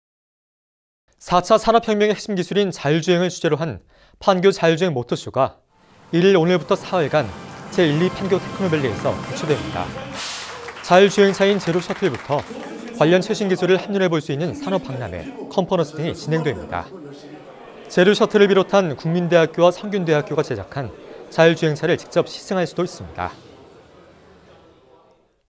제3회 판교 자율주행모터쇼 개최
4차 산업혁명의 핵심기술인 자율주행을 주제로 한 판교 자율주행모터쇼가 1일 오늘부터 사흘간 제1‧2 판교테크노밸리에서 개최됩니다.

자율주행차인 ‘제로셔틀’부터 관련 최신기술을 한눈에 볼 수 있는 산업박람회, 컨퍼런스 등이 진행됩니다.

제로셔틀을 비롯한 국민대학교와 성균관대학교가 제작한 자율주행차를 직접 시승할 수도 있습니다.

영상취재 : 이효진 , 영상편집 : 김상환